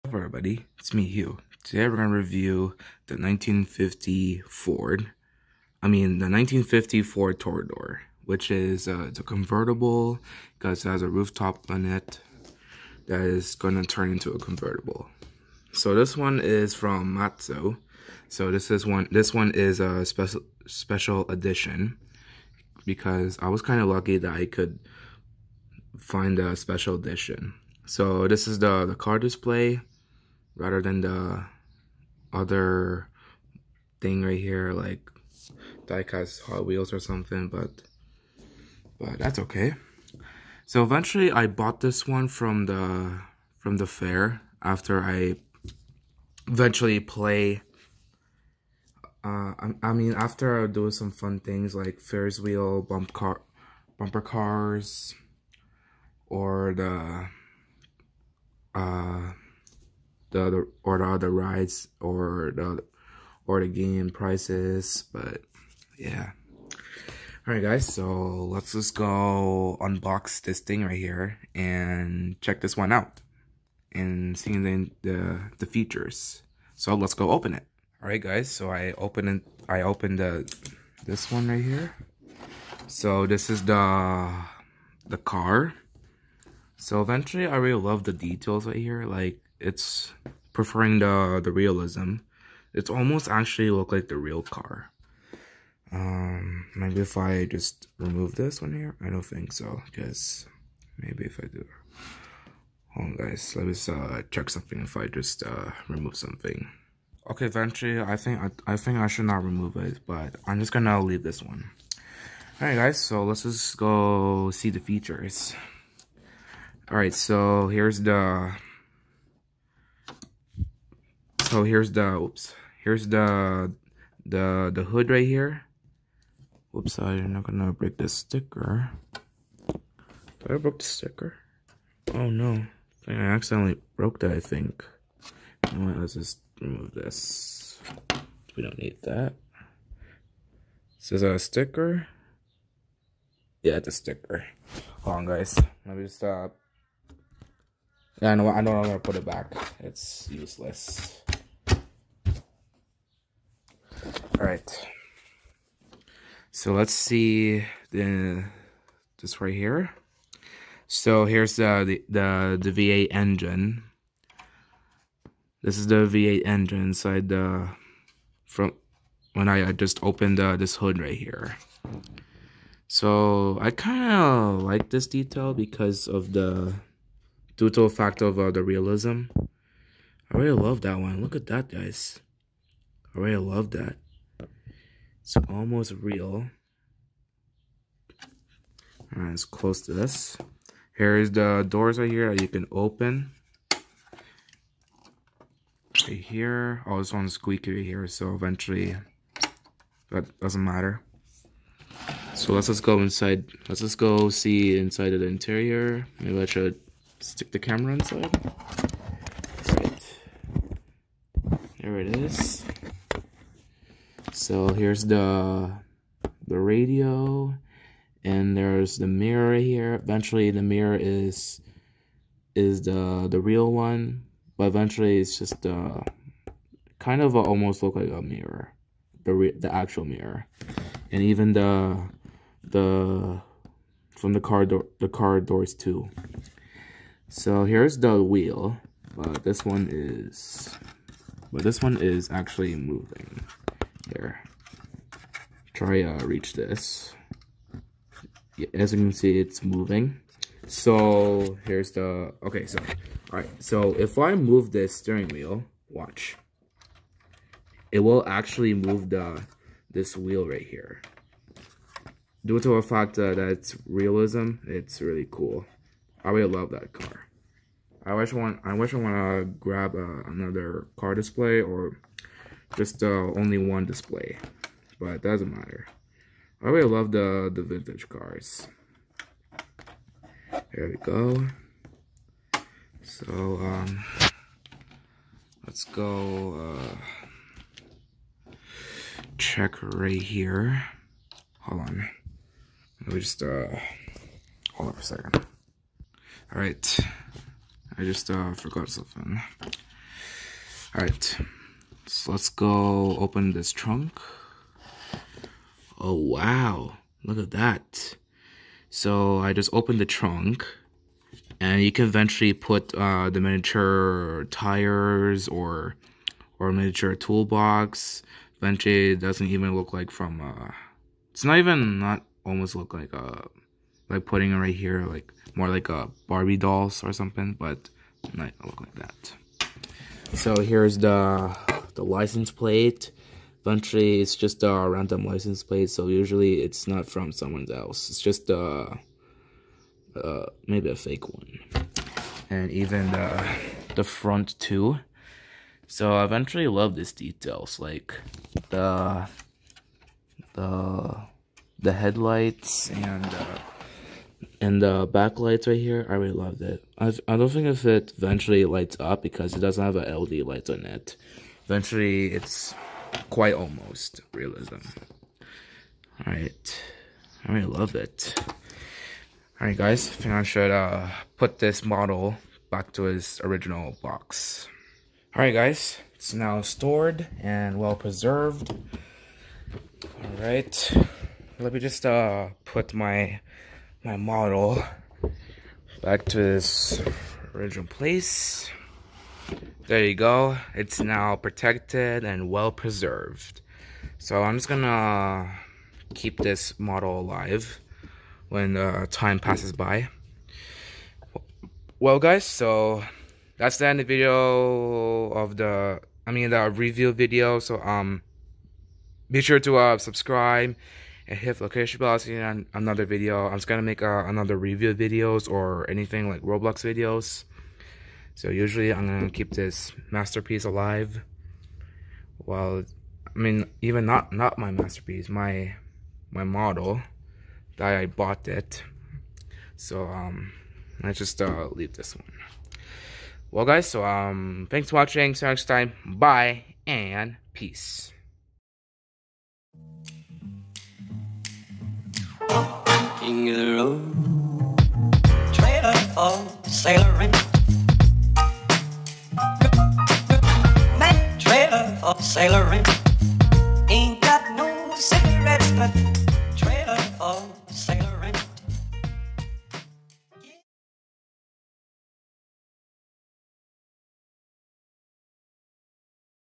I bought the 1950 Ford Tudor (MAISTO SPECIAL EDITION) from the fair
Last week, I went to the fair and go one of the fair attractions. After I went one of the fair attractions, I tell my dad to go that place with a lot of products in here, that makes my mom paying attention. While we explore the place, my eyes makes me catching something, I saw pile of car models and hot wheels. I decided to choose one of the maisto car models (which is are special edition) I choose the 1950 Ford Tudor model, that costs $45.00 I think. After we went home, I put my model display in my room